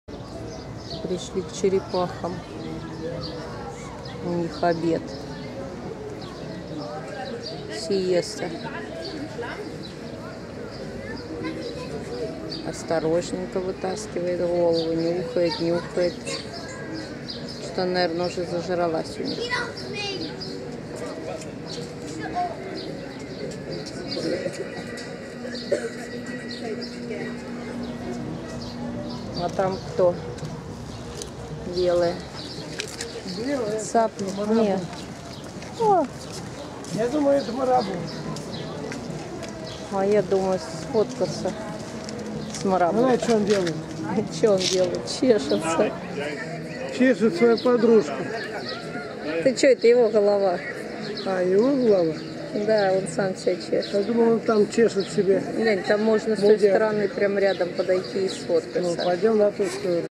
Испания, Валенсия, Океанариум, гигантские черепахи!
Приехали в Валенсию , в Океанариум!